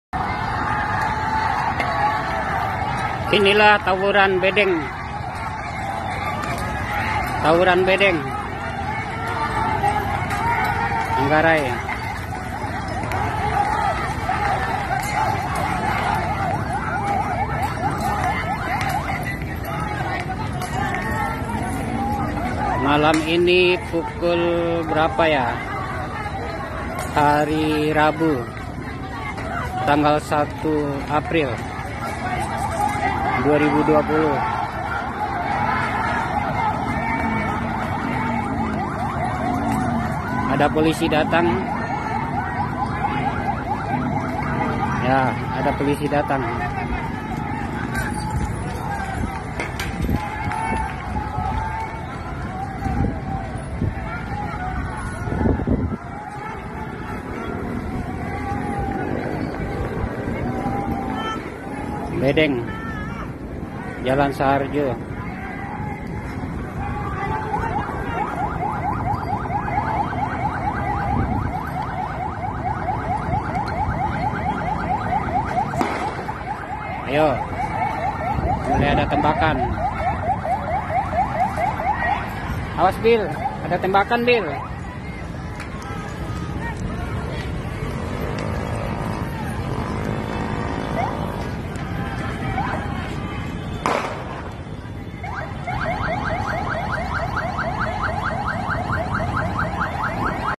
Miris ditengah Wabah corona malah tawuran warga jakarta
Saat wabah virus corona tengah mendera Indonesia, ternyata tak menghentikan tawuran warga yang terjadi di daerah langganan tawuran yakni Manggarai, Jakarta Selatan.

Video tawuran warga yang terjadi Rabu (1/4/2020) malam ini, dibakikan oleh akun instagram yang langsung beredar viral.

Unggahan video itu kemudian diberi judul atau caption: "Tawuran warga terjadi rabu malam ( 01/04/03 ) di kawasan Jalan Bedeng ( Dr. Saharjo ) Manggarai - Tebet sekitar pukul 20.10 WIB..belum diperoleh informasi sebab dan akibat pecahnya tawuran tersebut..

Dalam video yang dibagikan itu, terlihat ratusan warga terlibat saling serang dan suasana kacau, bahkan bisa jadi para 'peserta' tawuran itu tidak mematuhi phsycal distance atau 'Jaga Jarak Fisik/Badan'.